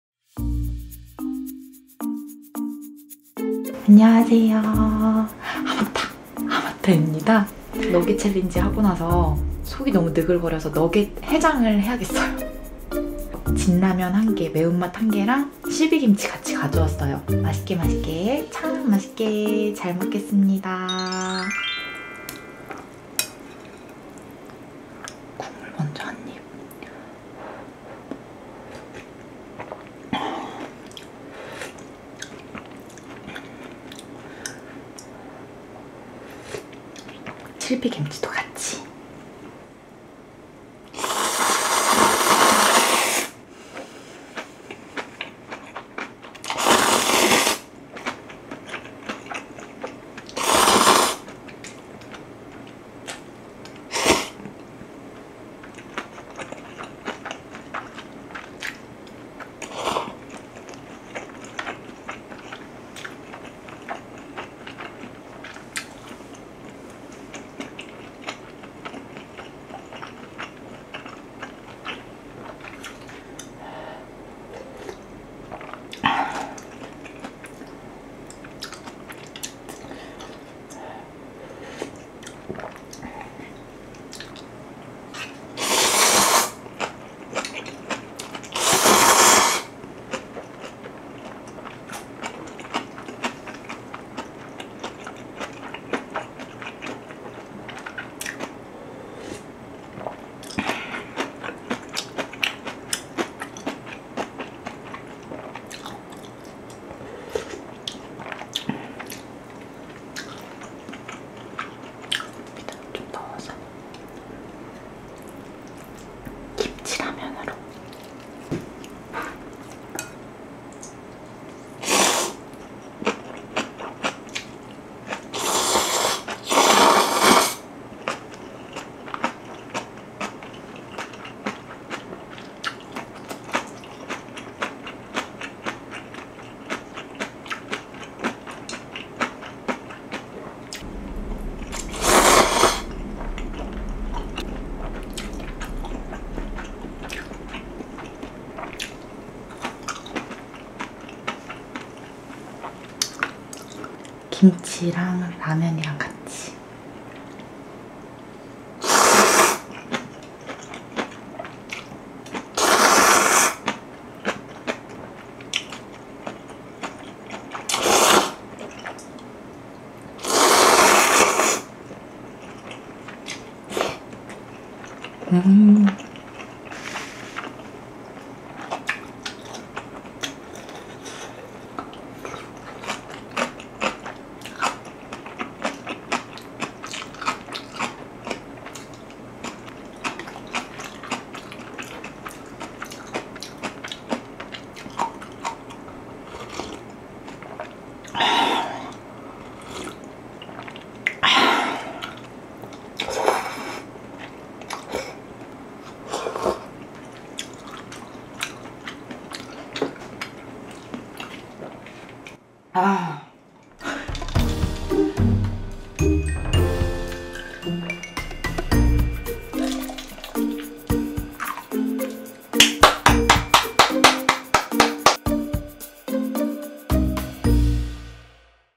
매운진라면과 실비김치로 너겟 해장 먹방 MUKBANG
느글느글한 속 달래는데는 라면에 김치만한게 없죵:)
간단히 한그릇만 할게용:)